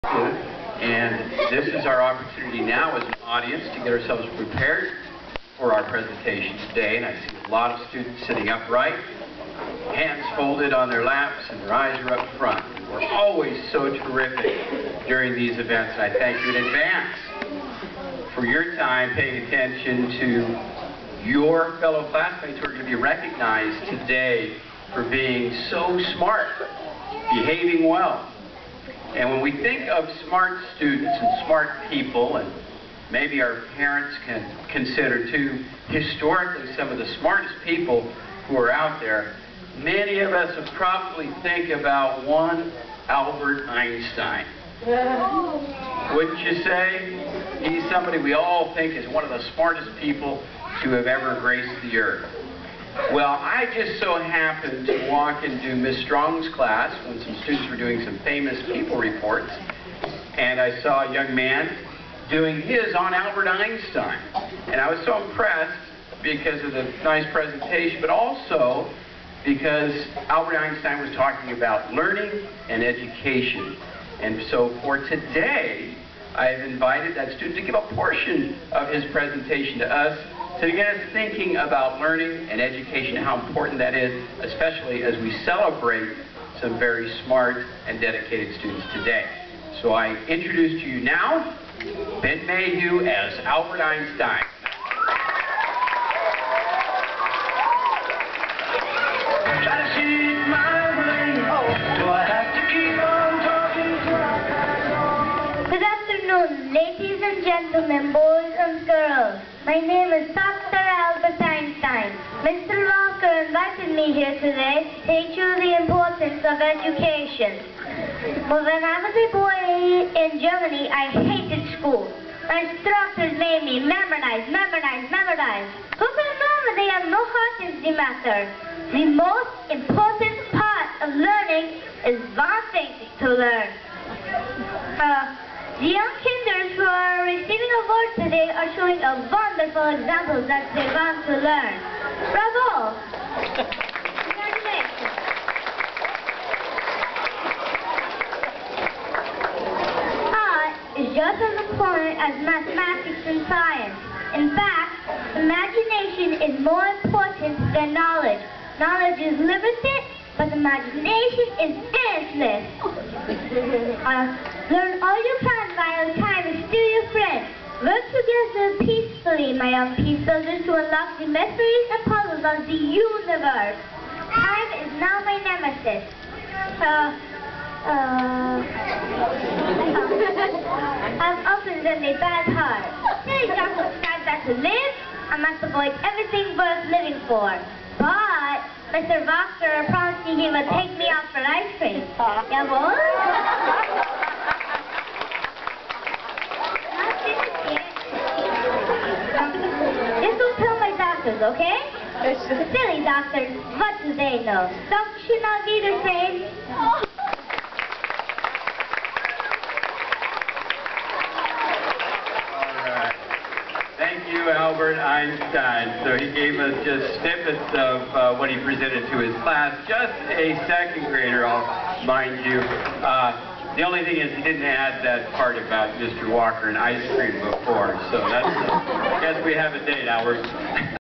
Ben as Einstein
2nd grader Ben performs as Einstein for school academic awards ceremony.